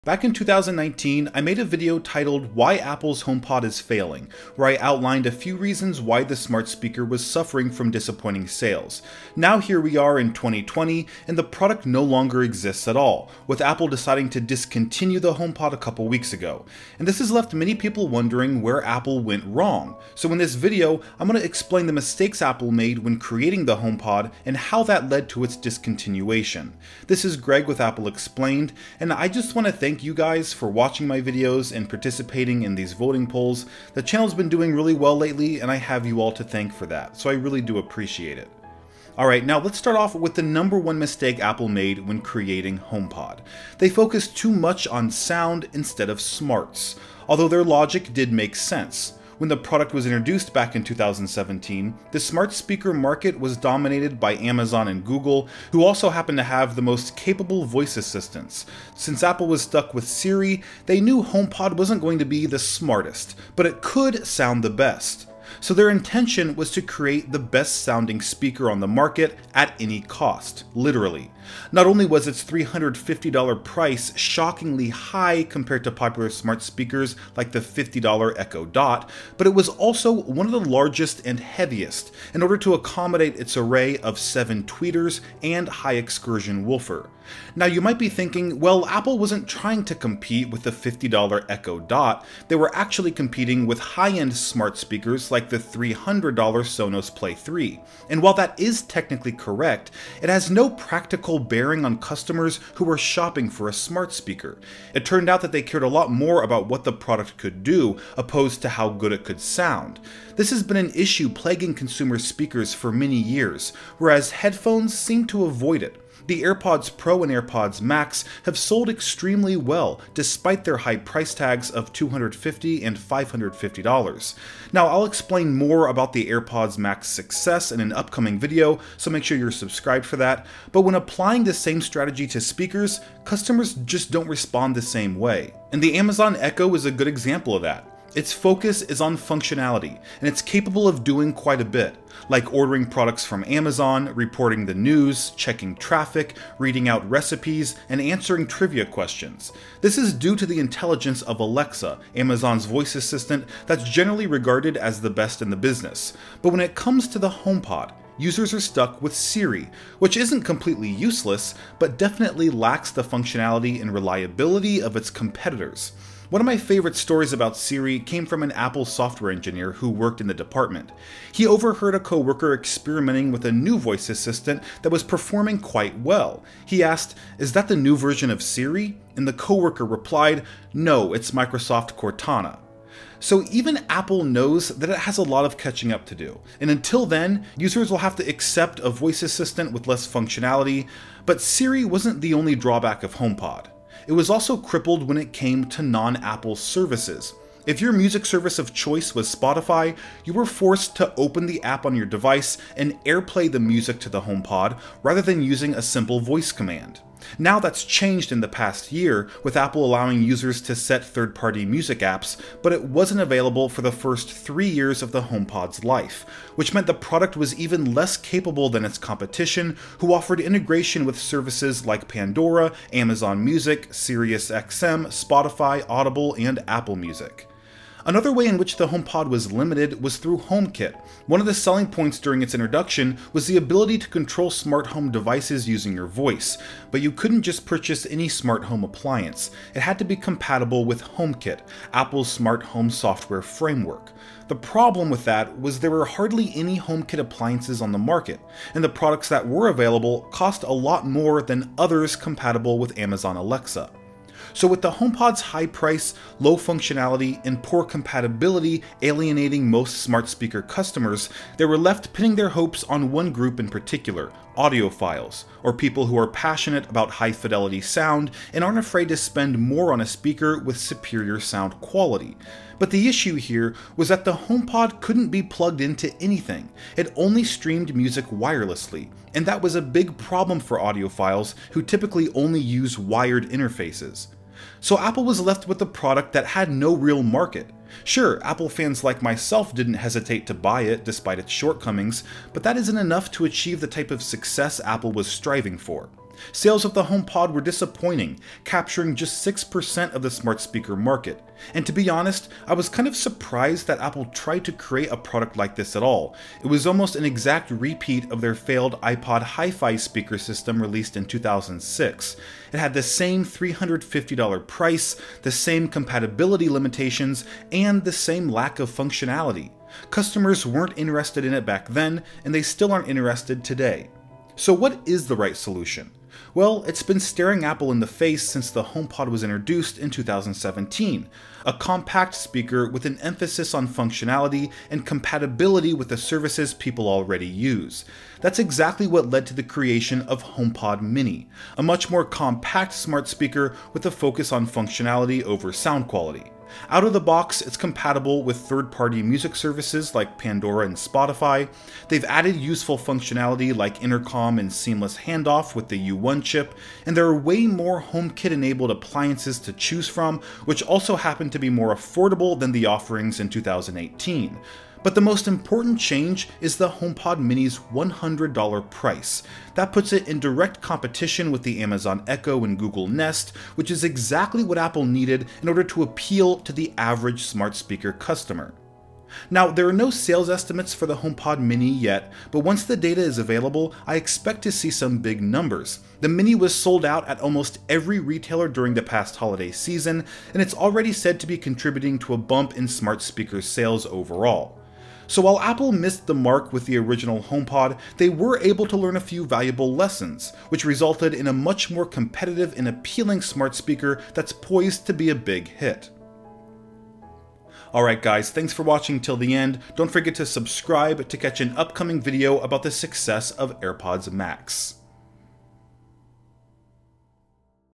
Apple's HomePod Failure
Back in 2019 I made a video titled “Why Apple’s HomePod Is Failing,” where I outlined a few reasons why the smart speaker was suffering from disappointing sales. Now here we are in 2021, and the product no longer exists at all. With Apple deciding to discontinue the HomePod a couple weeks ago. And this has left many people wondering where Apple went wrong. So in this video, I’m going to explain the mistakes Apple made when creating the HomePod, and how that led to its discontinuation.